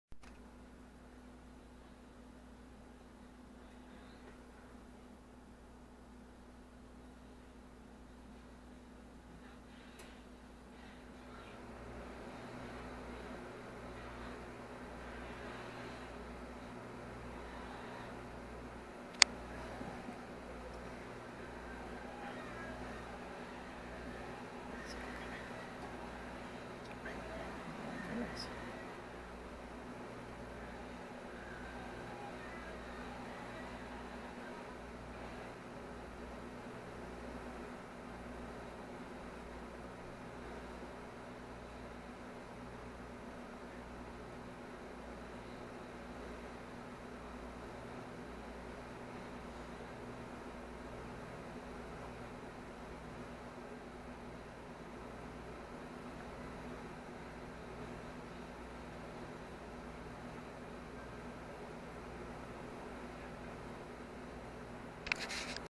Fennec Listening to a Loud Movie in the Other Room
Fennec on my head